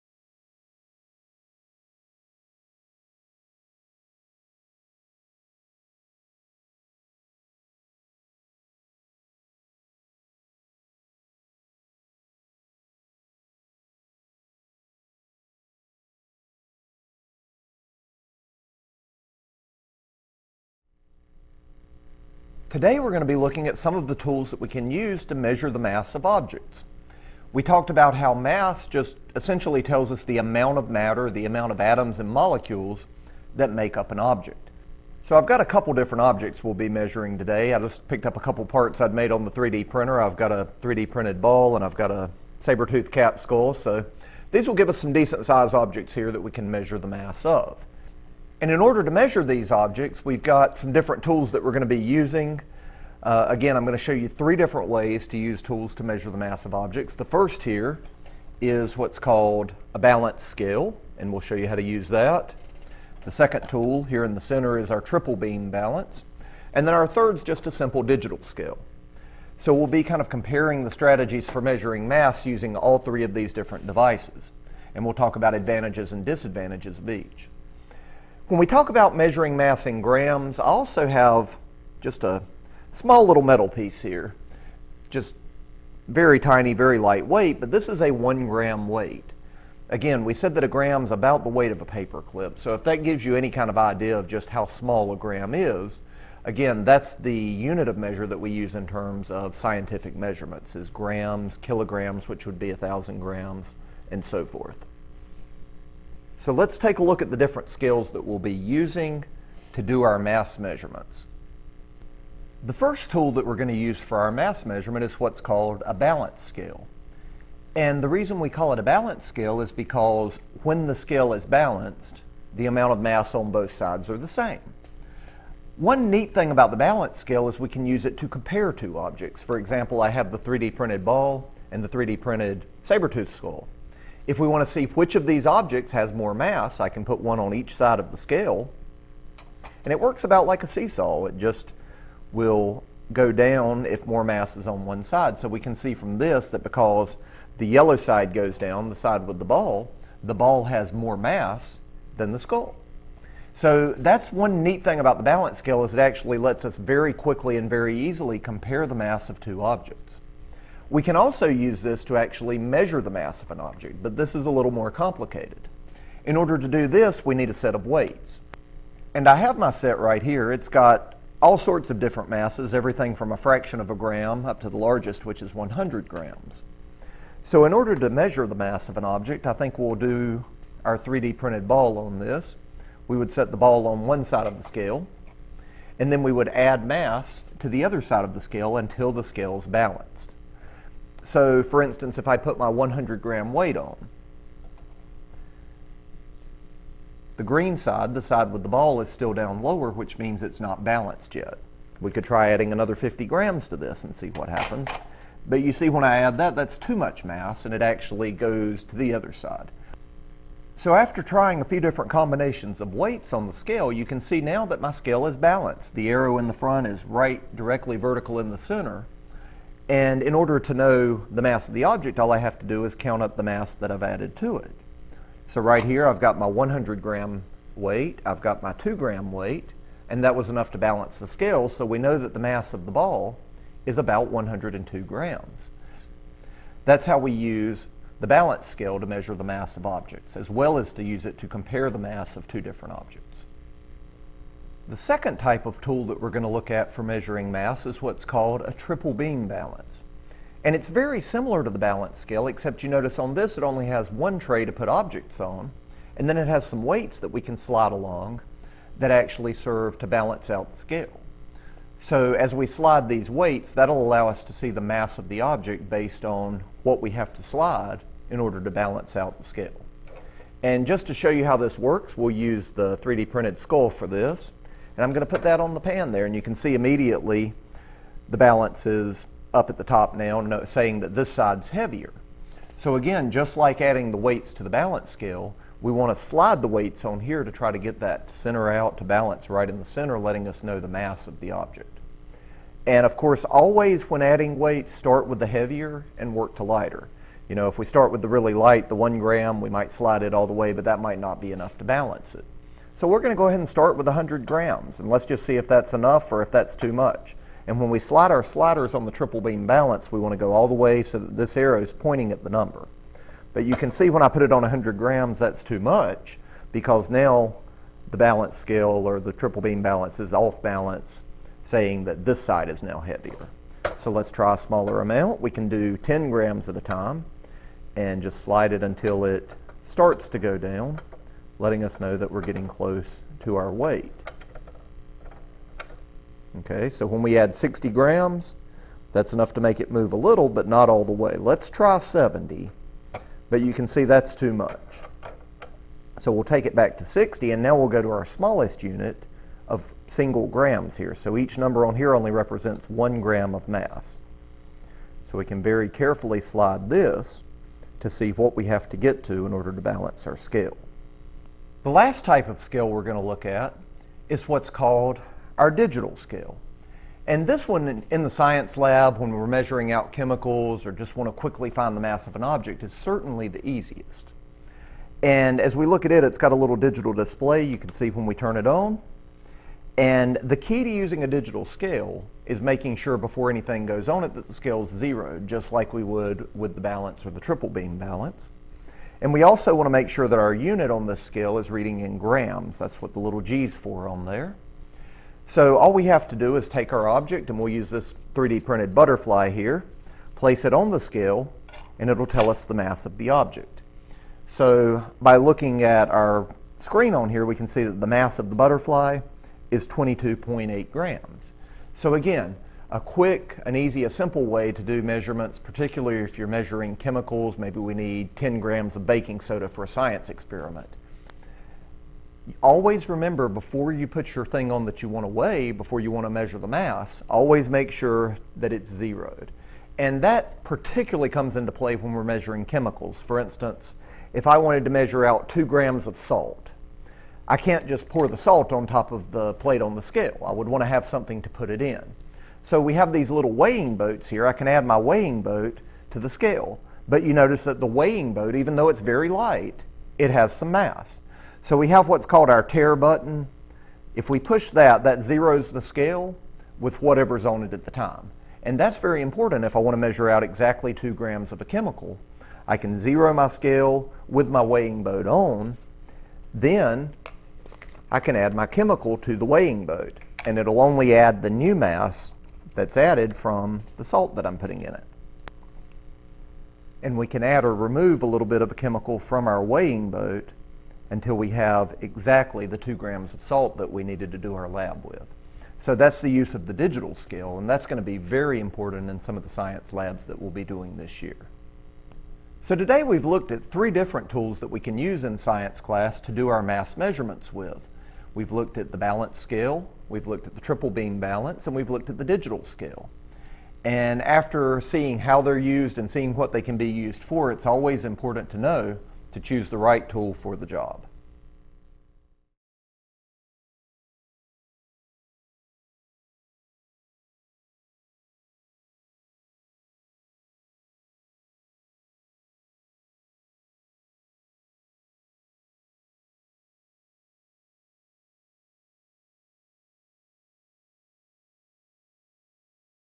Activity 5.1.1.A - Measuring an Object's Mass
In lesson 1, we learned that matter is anything that has mass and takes up space. In this activity we explore three different methods of measuring an object's mass using a balance scale, a triple beam balance, and a digital scale.